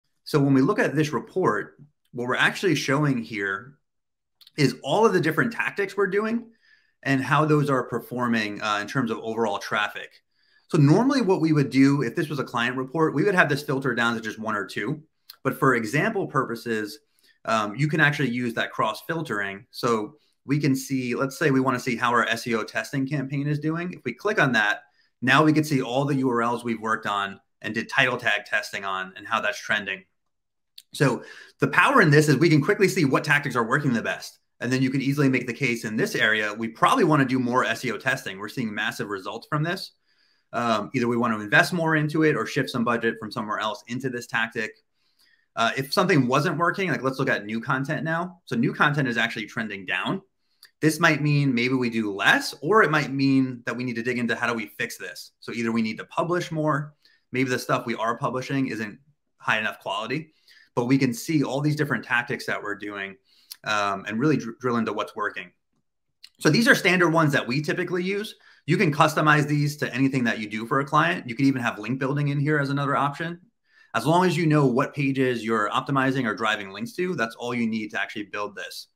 SEO Tactic Reporting In Looker Studio (Learn What Is Working The Best)
Reporting on the specific SEO tactics you are doing for clients is a great way to understand where most of your results are coming from.

You can use this report to understand which tactics are worth investing more into and which ones either need to be improved or shifted out of.

We use Looker Studio to customize this report based on the exact scope our clients have. It's been a big help when we need to have data to support why we are recommending budget changes.